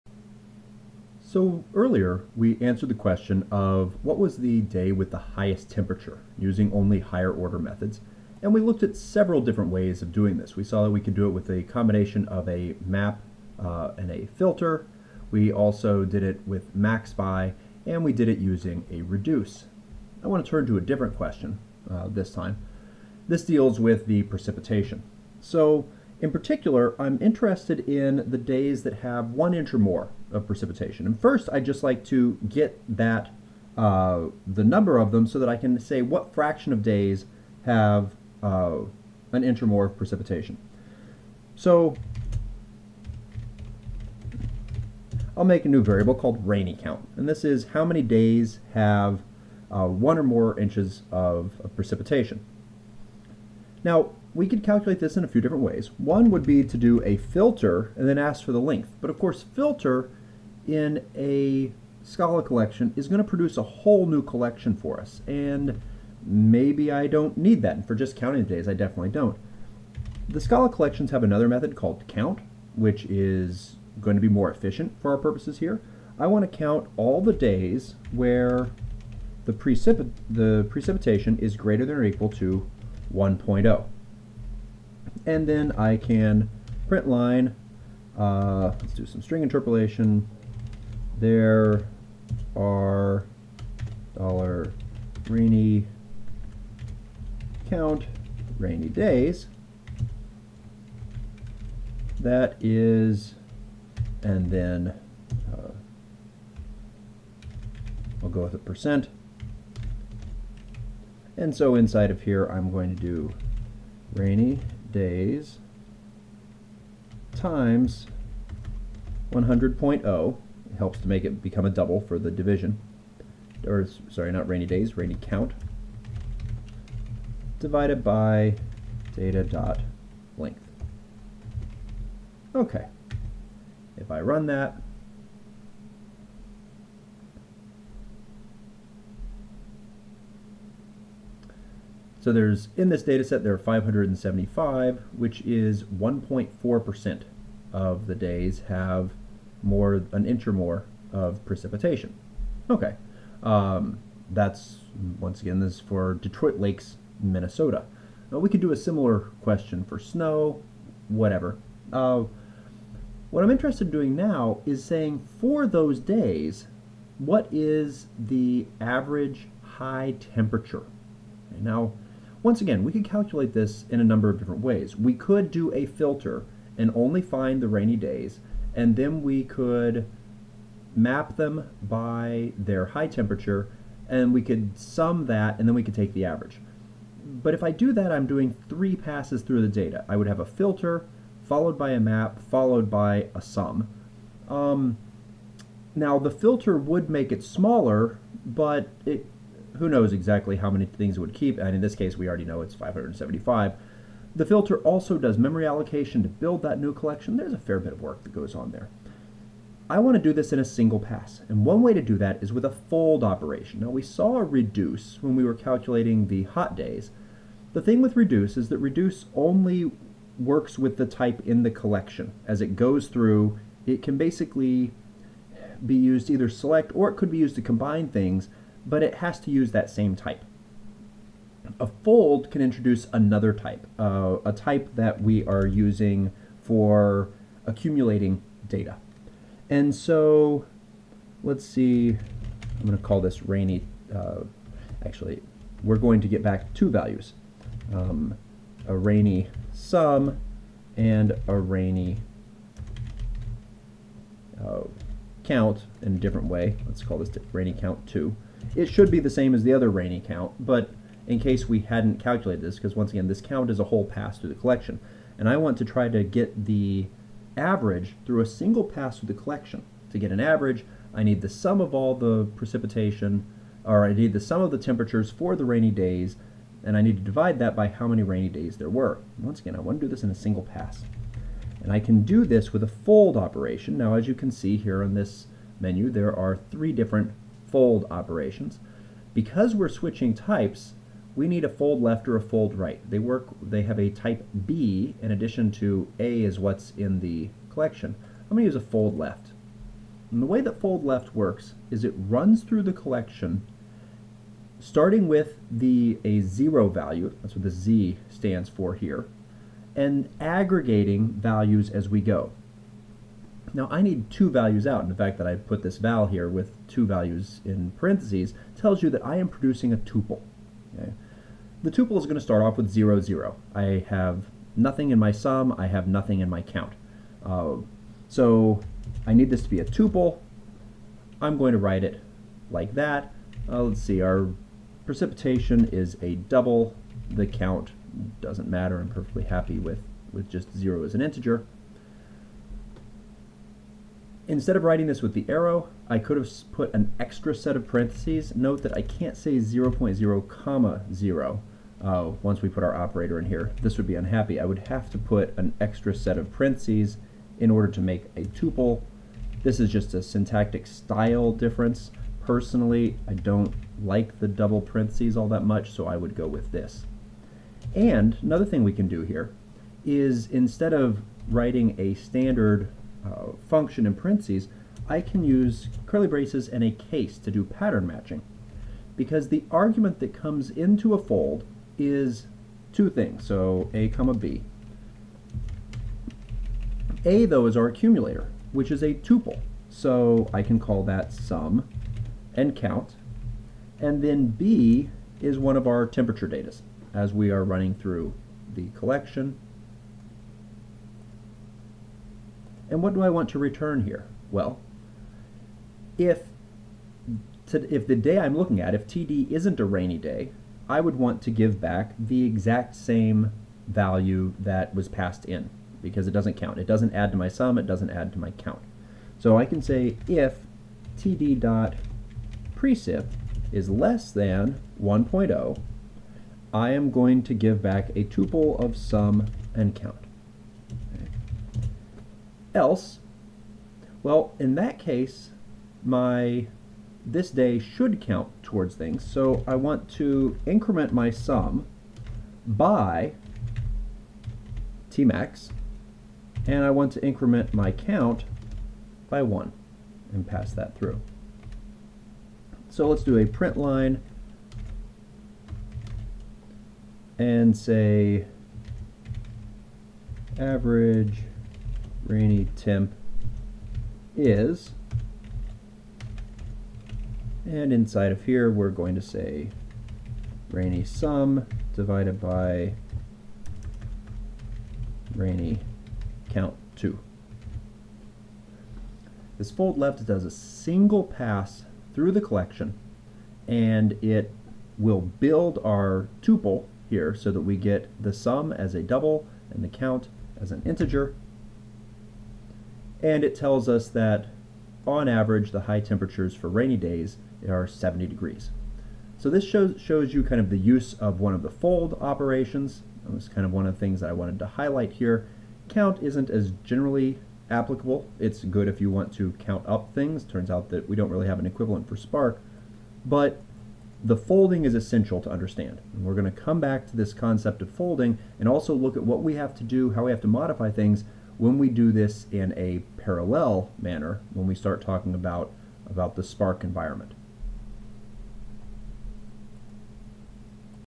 Data Processing with Higher-order Methods Part 4 (Scala)
In this video we look at calculating the average temperature on certain days using a fold operation.